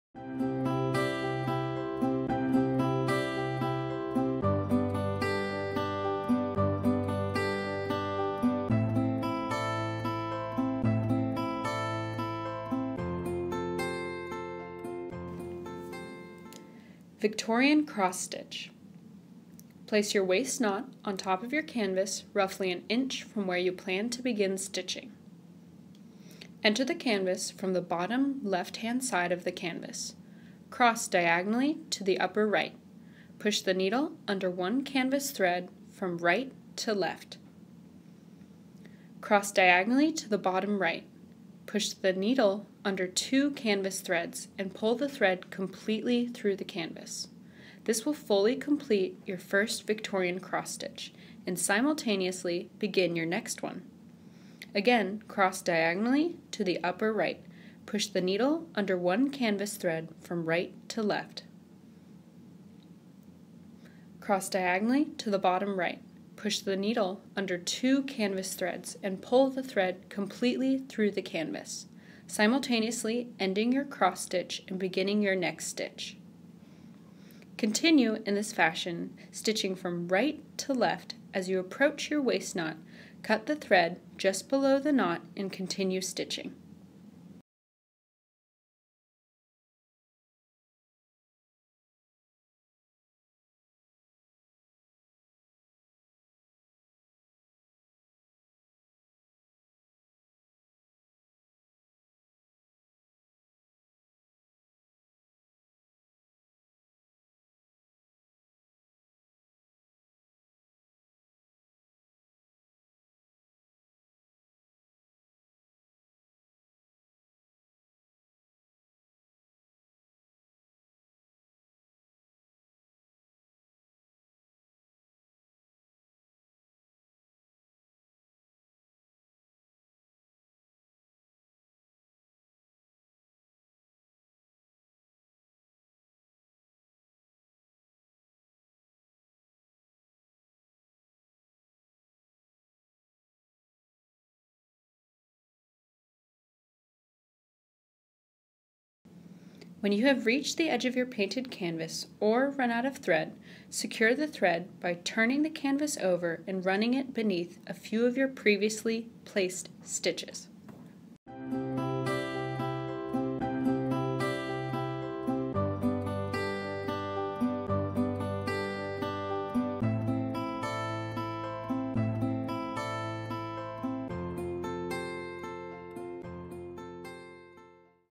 How to Victorian Cross Stitch - Right Handed - Elizabeth Bradley
Elizabeth Bradley needlepoint kits suggest the Victorian Cross Stitch as the preferred stitching method. Here is a visual demonstration of the needlepoint stitch, which provides unparalleled durability to your needlepoint kit, creating an heirloom to last generations.